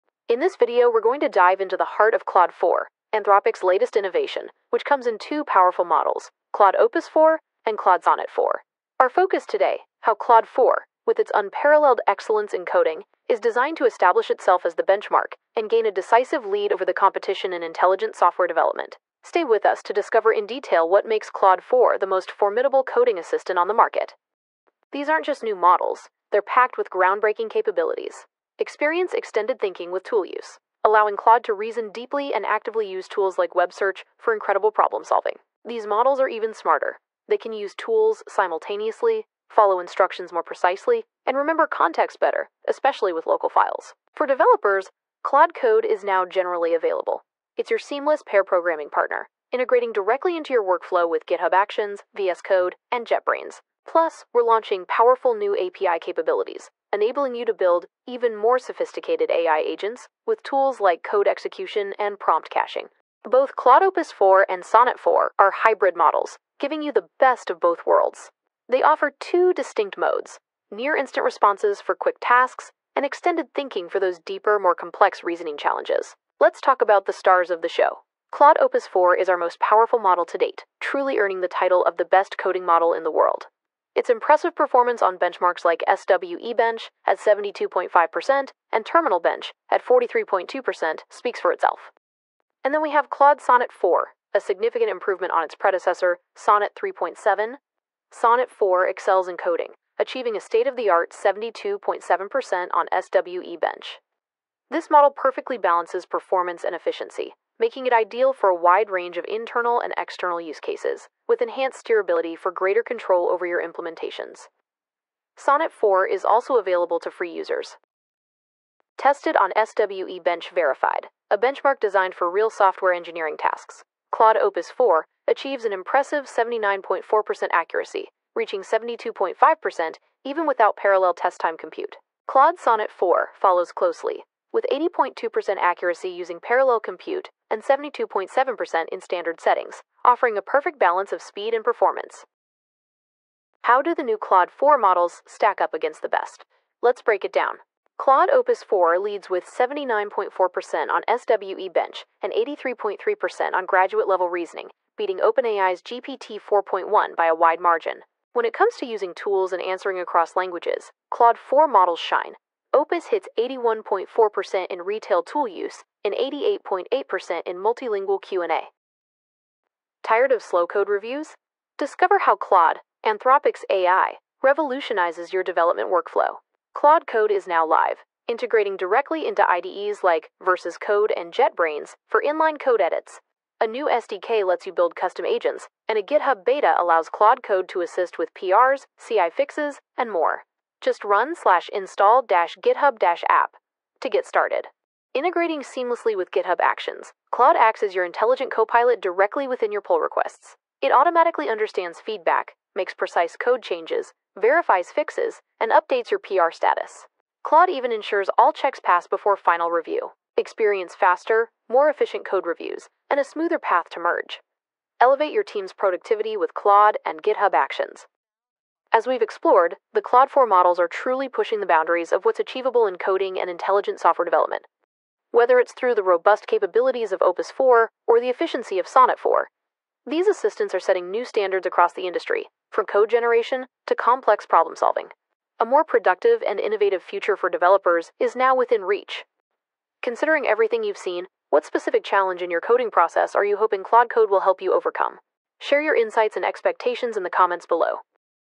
Claude 4 SMASHES Coding Records: ChatGPT Beaten!
Could Claude 4 be the Al coding revolution we've been waiting for? Discover what makes Anthropic's new Claude 4 Al model truly unique and how YOU can use it today to enhance your kflow and creativity. In this in-depth video, I break down its key features, discuss its potential impact, and provide a step-by-step guide to get started with Claude 4 right now.

 00:00 Introduction of Claude 4
 00:24 Groundbreaking Capabilities
 00:43 Seamless pair programming
 01:14 Claude Opus 4 benchmarks
 01:31 Claude Sonnet 4 Performance
 03:15 Integration with GitHub Actions
 03:34 Conclusion